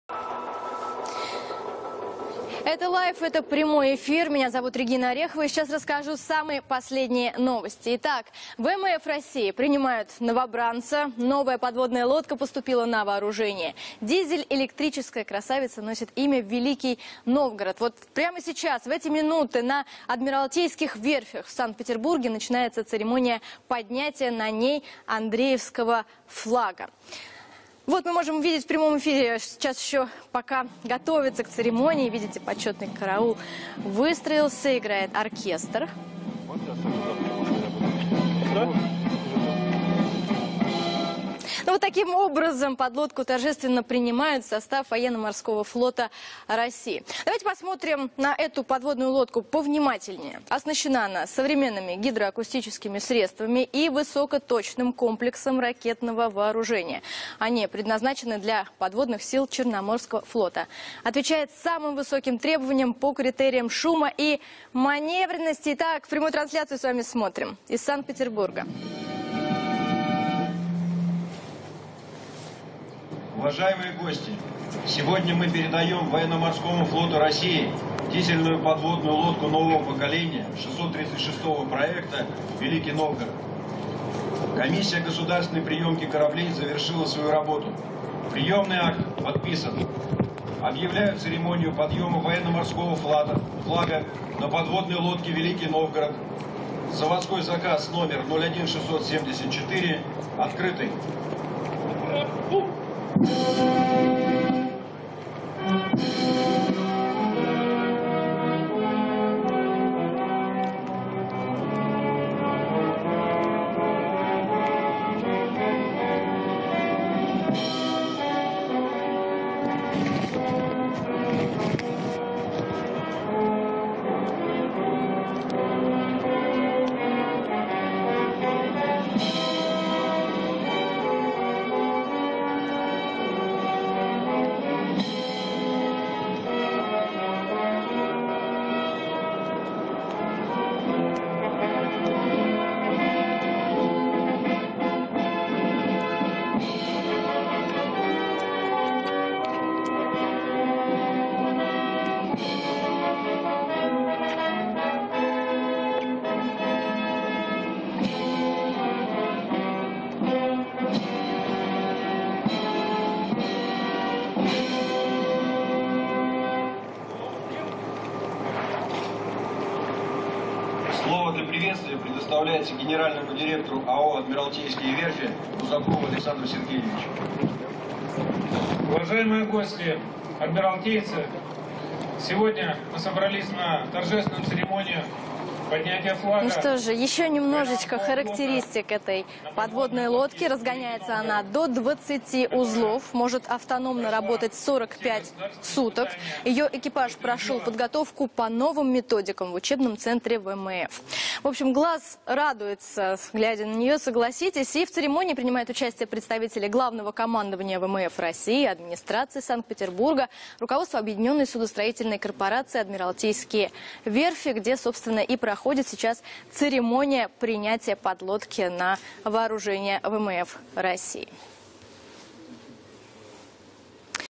ВМФ России получит новую подлодку
Новая подводная лодка поступила на вооружение ВМФ России